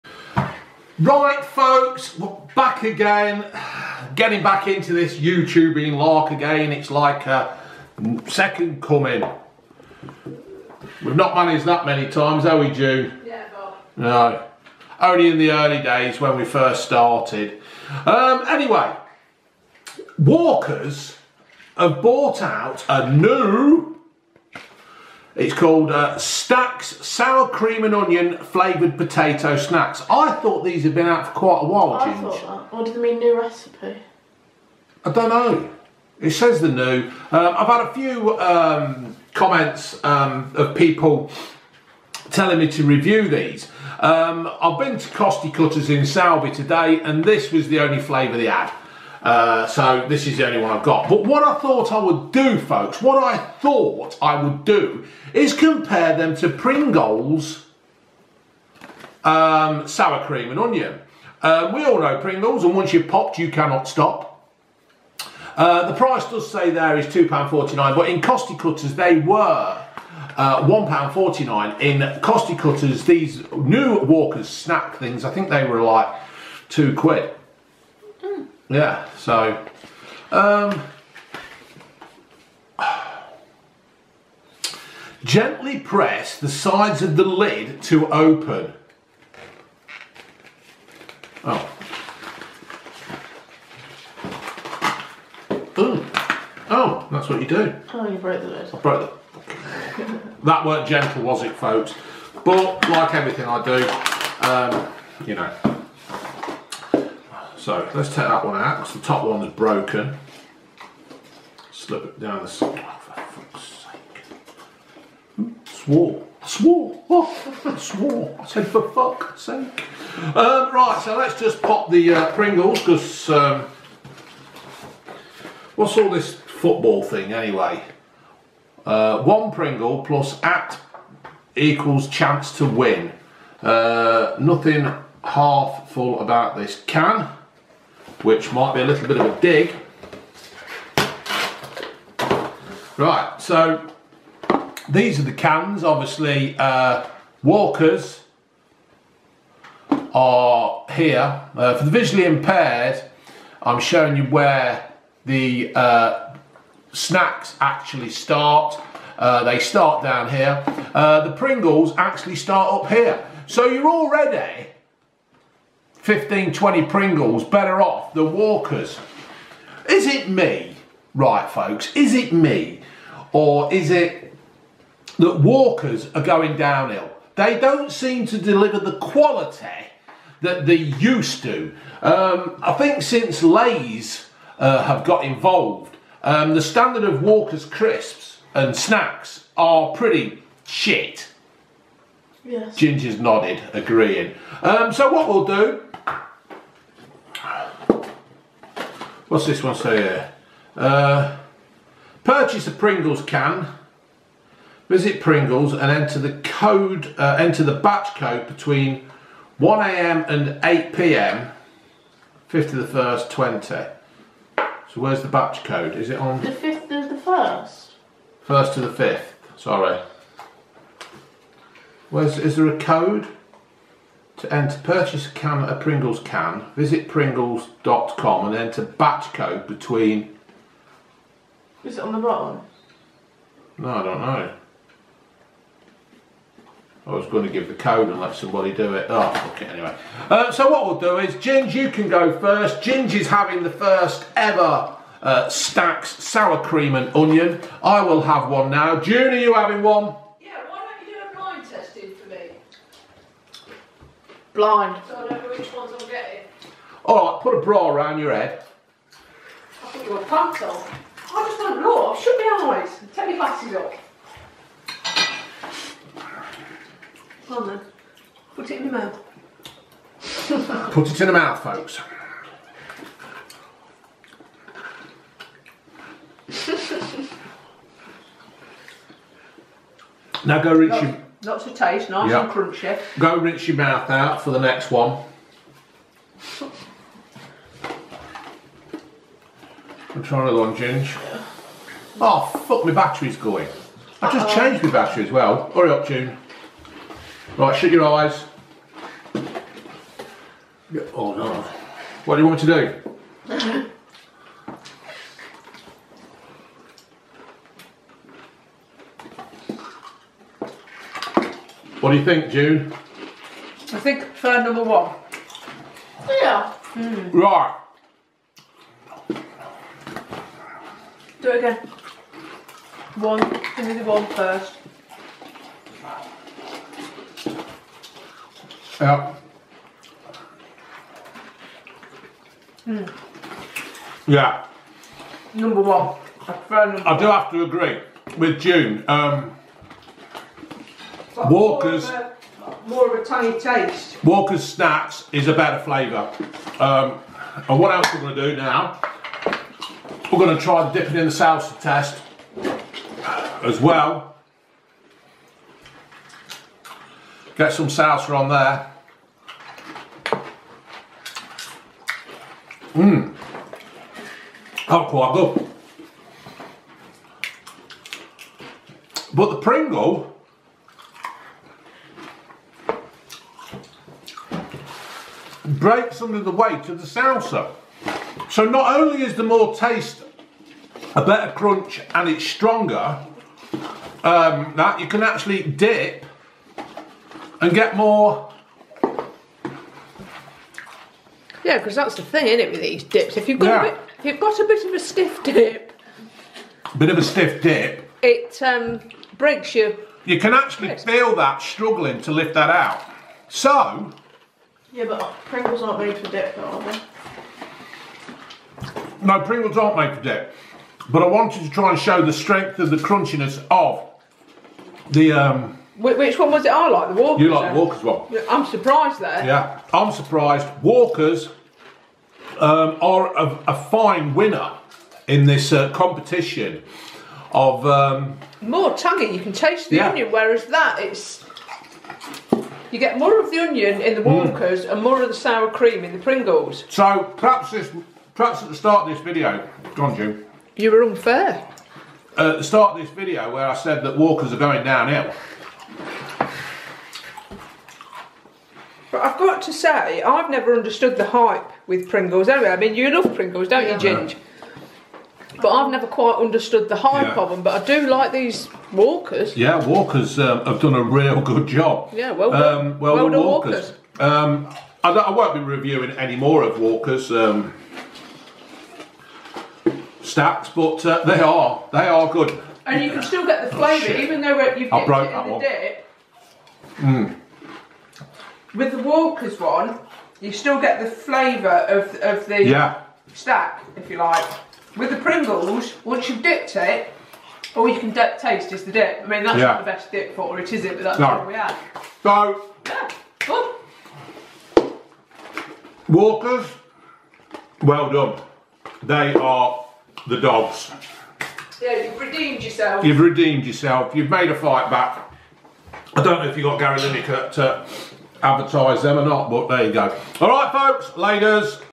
Walkers Stax Sour Cream & Onion V's Pringles Sour Cream & Onion Food Review.
So Walkers have a rival to Pringles out and here is what me, June and Ginger think of this battle of the tubes.